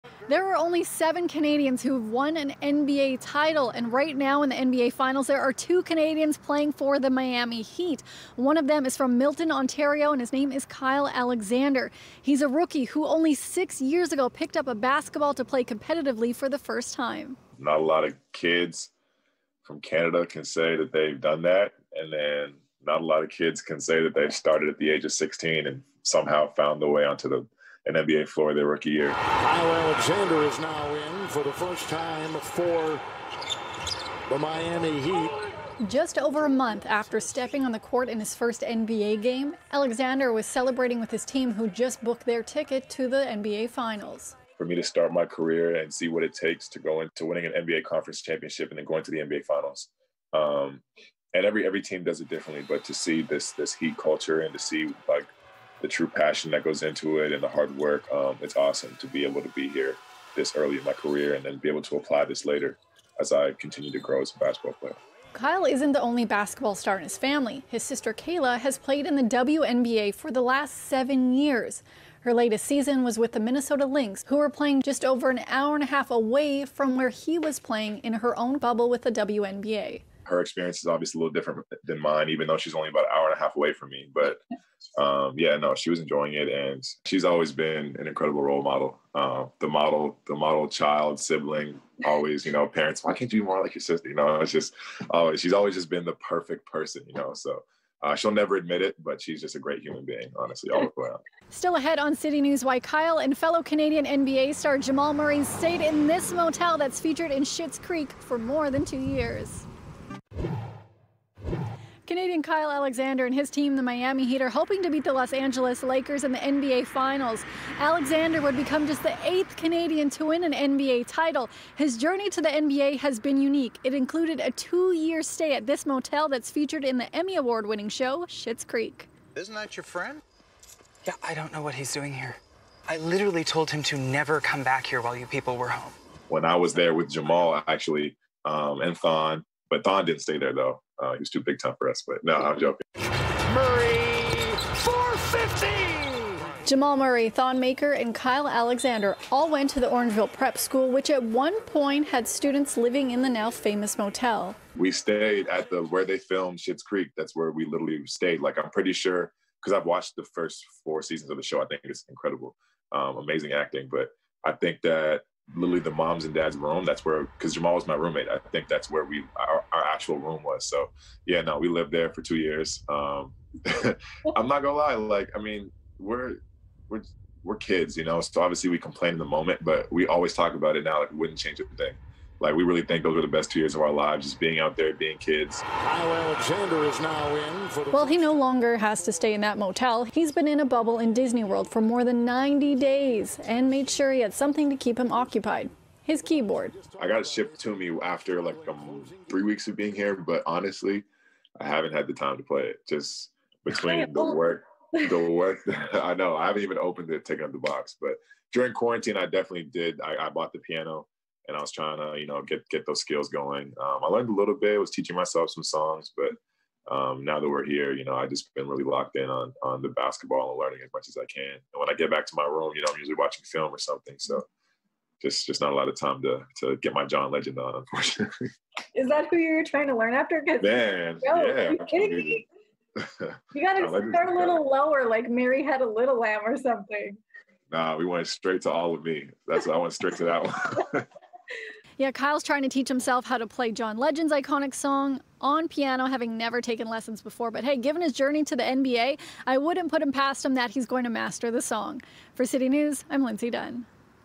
Canadian NBA rookie Kyle Alexander soaking in NBA Finals experience
Canadian NBA player Kyle Alexander is soaking in the NBA Finals experience. The rookie speaks to Lindsay Dunn about his journey to the league and why he and Jamal Murray at one point lived in the motel featured in Schitt's Creek.